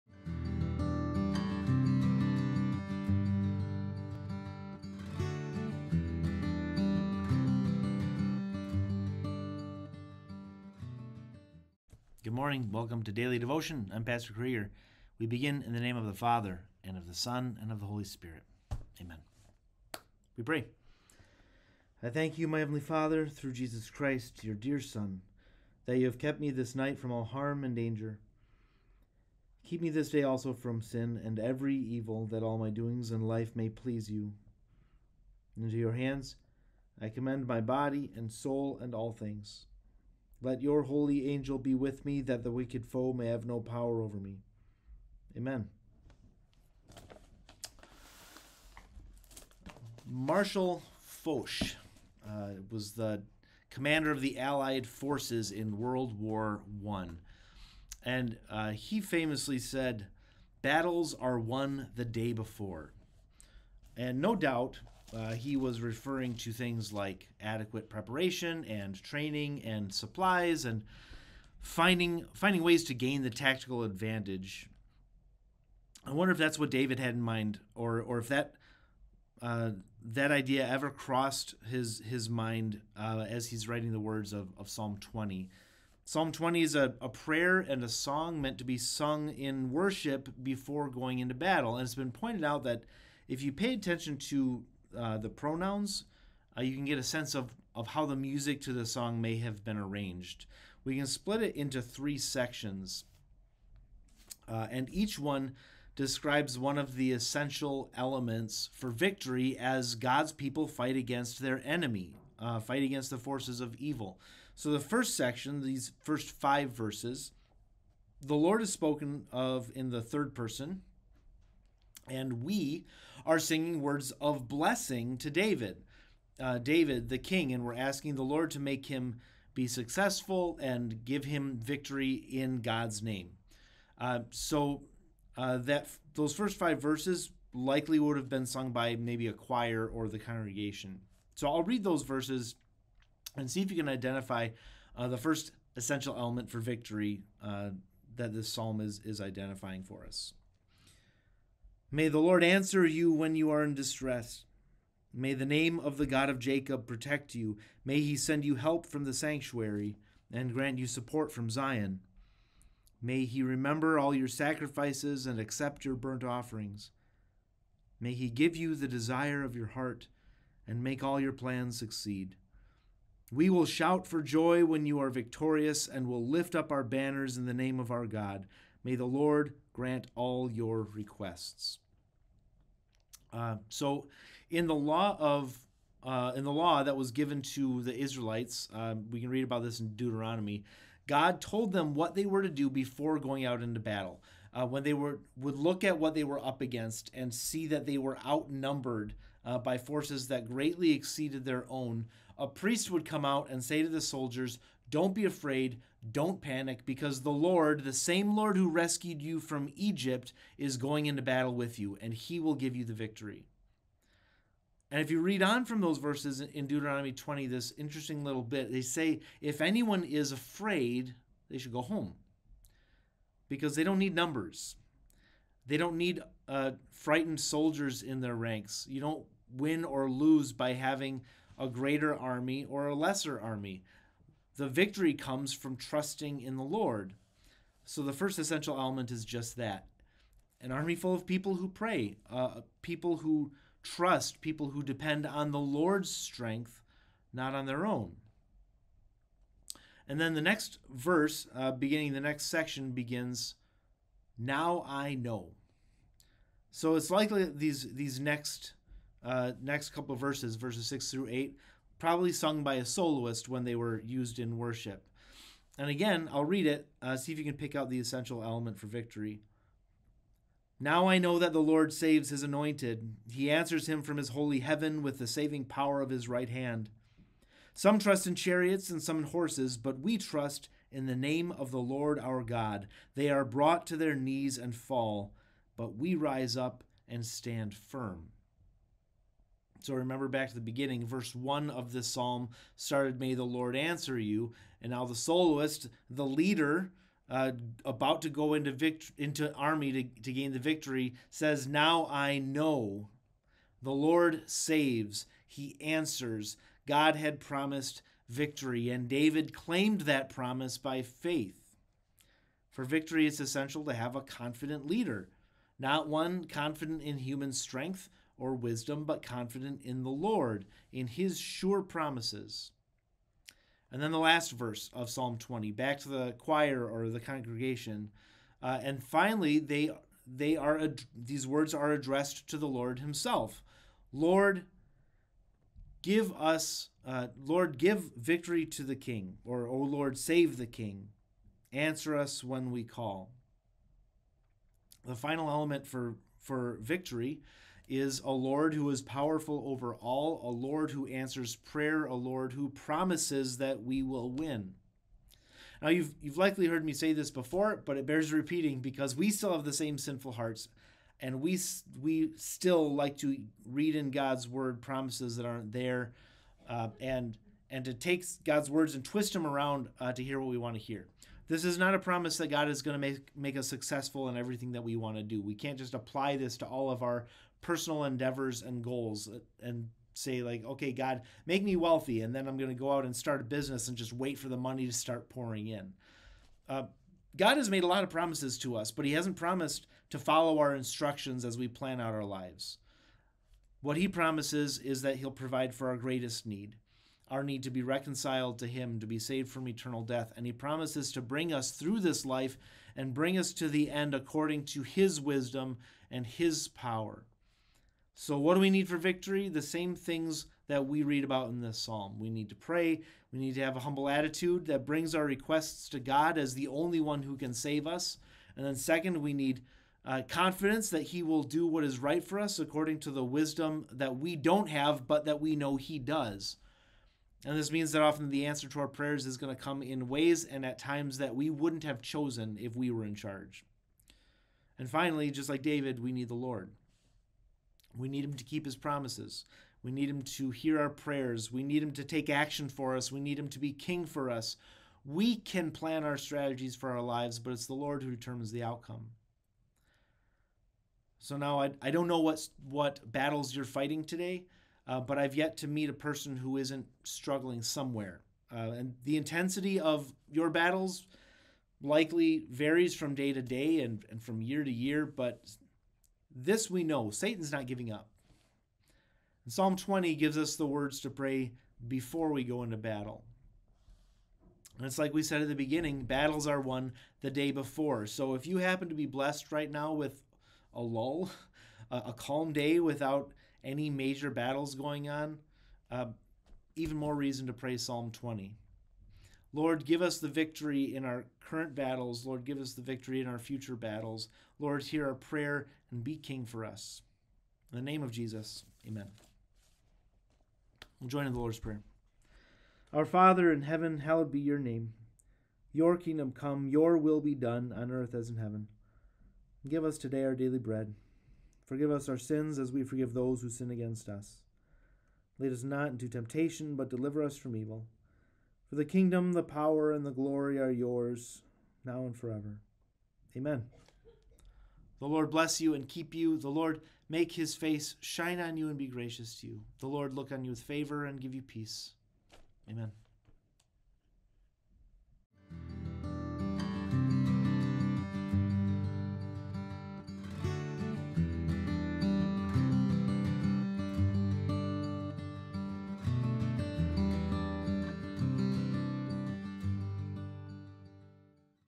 Daily Devotion - Monday, February 6, 2023
Psalm 20 is a prayer in time of war, and as we read it we will identify the three things we need to be victorious.

1 May the Lord answer you when you are in distress;
    may the name of the God of Jacob protect you.
2 May he send you help from the sanctuary
    and grant you support from Zion.
3 May he remember all your sacrifices
    and accept your burnt offerings.
4 May he give you the desire of your heart
    and make all your plans succeed.
5 May we shout for joy over your victory
    and lift up our banners in the name of our God.

May the Lord grant all your requests.

6 Now this I know:
    The Lord gives victory to his anointed.
He answers him from his heavenly sanctuary
    with the victorious power of his right hand.
7 Some trust in chariots and some in horses,
    but we trust in the name of the Lord our God.
8 They are brought to their knees and fall,
    but we rise up and stand firm.
9 Lord, give victory to the king!
    Answer us when we call!